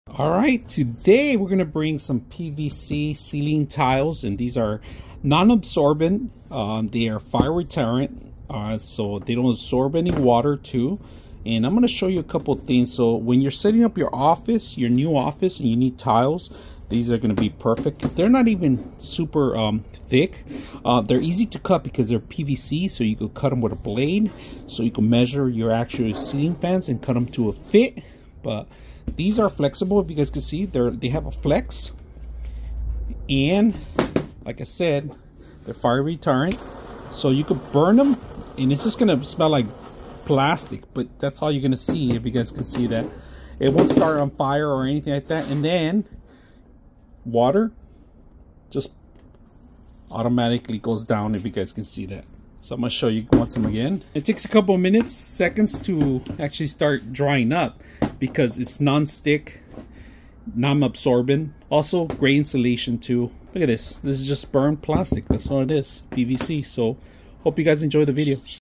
Noooza 2ft x 2ft Drop Ceiling Tiles
Noooza 2ft x 2ft Drop Ceiling Tiles，Smooth White PVC Ceiling Panel 24 x 24in. Waterproof, Washable and Fire-Rated - Reusable - High-Grade PVC to Prevent Breakage-Package of 12 Tiles . Thank You